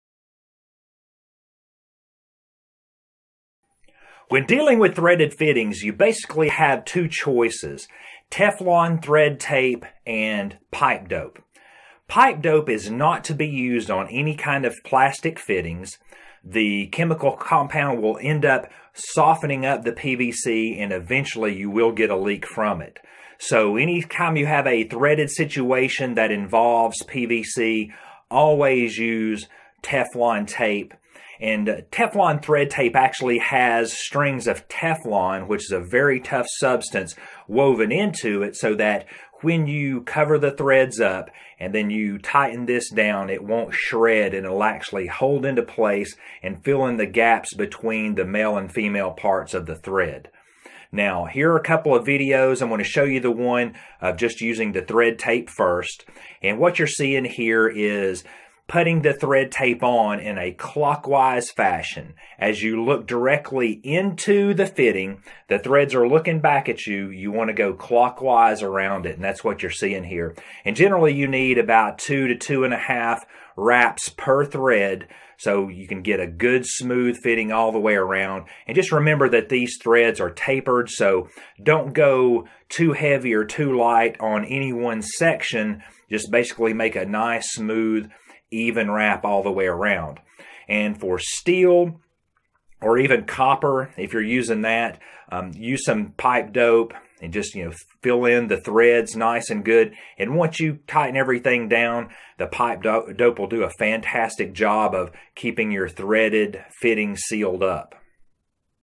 Using Teflon Tape or Pipe Dope (irrigation training)
Everything you need to know about using Teflon thread tape or pipe dope for irrigation uses.
This video is part of our online video training course Irrigation 101: Piping Systems.
Amazon Picks:
Teflon tape
Pipe Dope